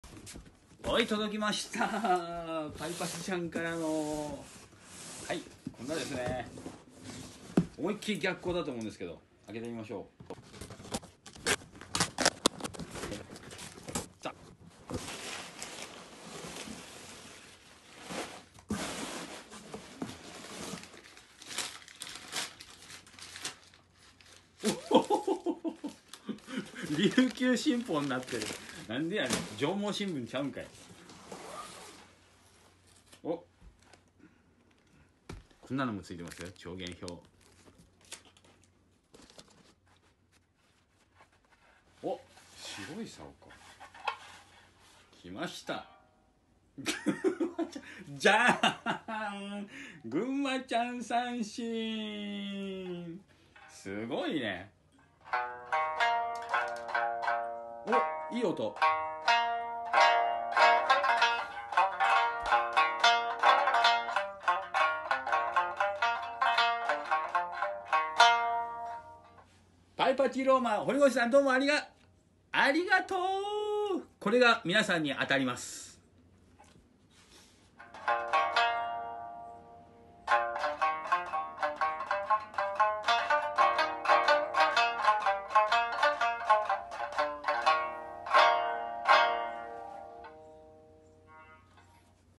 湘南三線音楽祭〜プレゼント三線編〜
湘南三線音楽祭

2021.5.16(日曜日)午後14時開演
藤沢市民会館小ホールにて
入場無料
Facebookの中継があります
三線、他グッズプレゼントあり！！

■三線、他グッズのプレゼントにつきましては、イベントの途中に一文字ずつを紹介し、それらをつなぎ合わせてできた単語を予想し、矢島三線工房に葉書で郵送してください。27日にスタッフが抽選をします。葉書の締め切りは5月21日の消印有効です。

ぐんまちゃん三線の提供は
🌺パイパティローマ関東三線店🌺
群馬県高崎市の事務所にて対応中〜
○三線販売、三線メンテナンス、修理
◆メール・LINEにて問合せ下さい